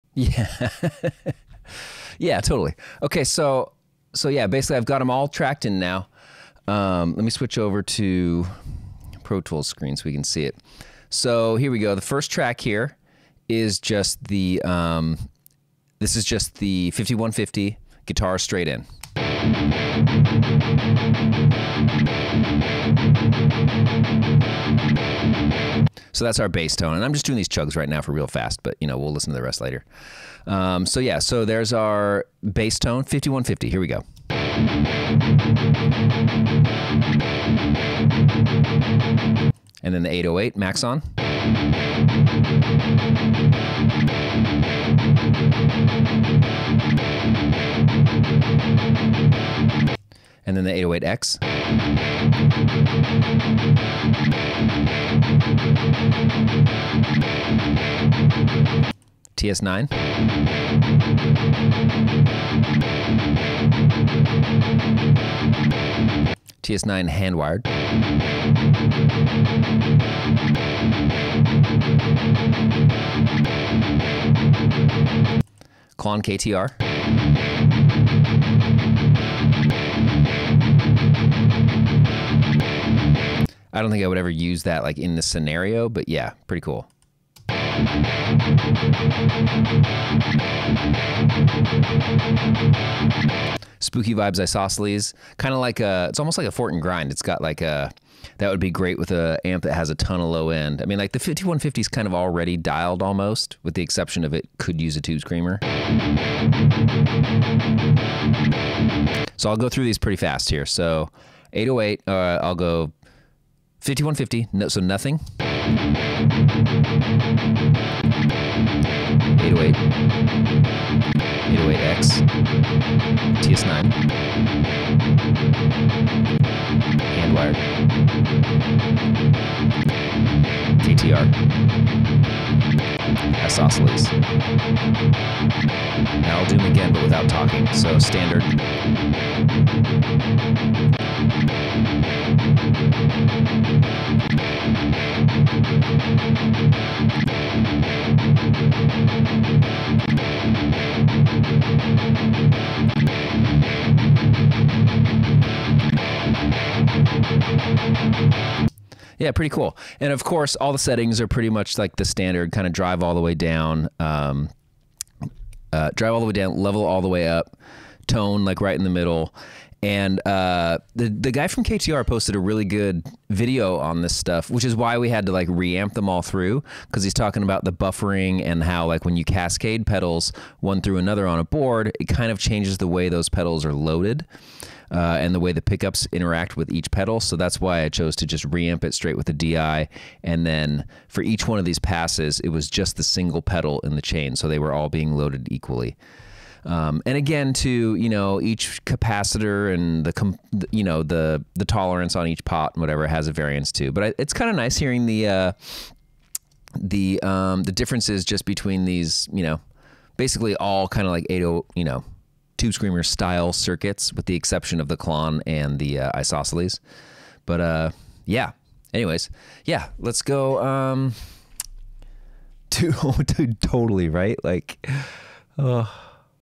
Different Kinds of Tube Screamers Compared
Beau Burchell compares different types of tube screamers circuits with a 5150iii live on his twitch channel. TS9, Maxon 808, Maxon 808x, TS9 HW, Klon KTR, Spooky Vibes Isosceles.